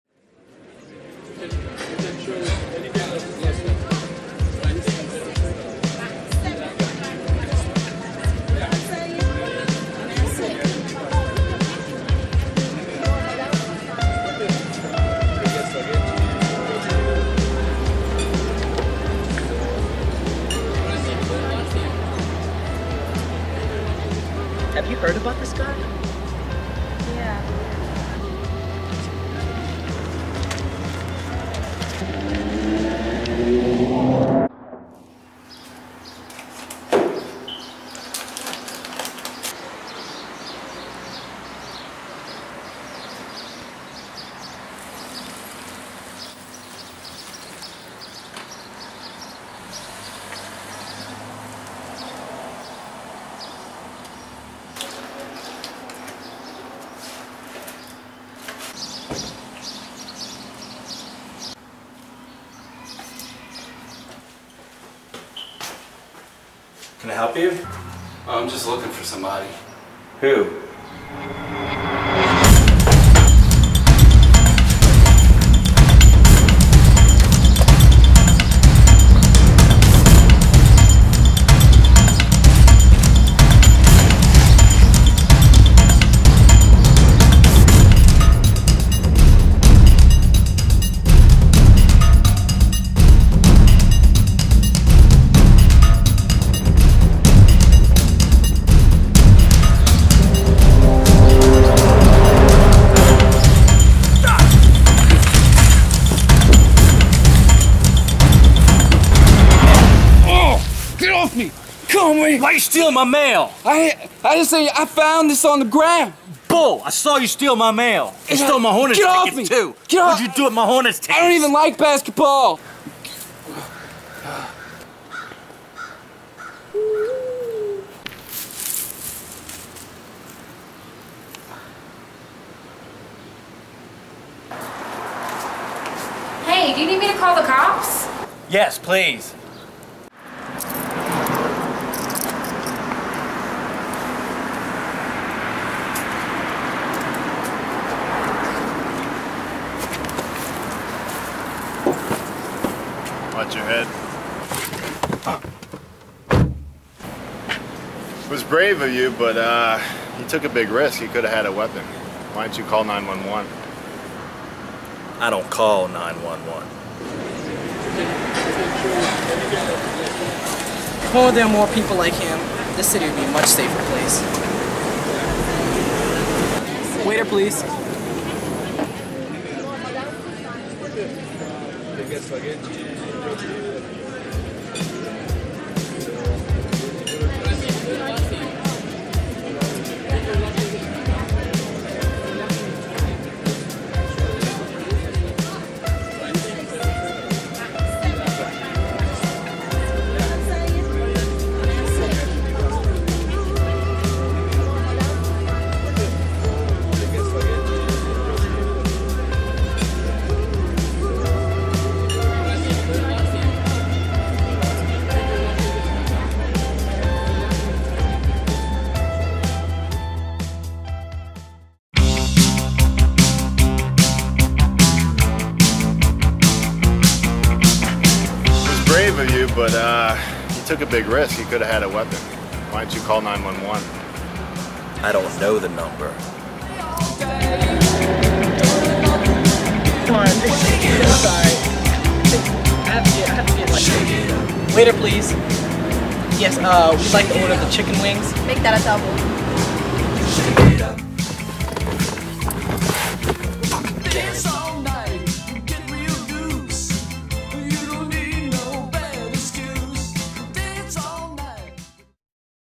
I Dont Call 911
Chicken Wing Re-enactment.  This reenactment is based on a true story.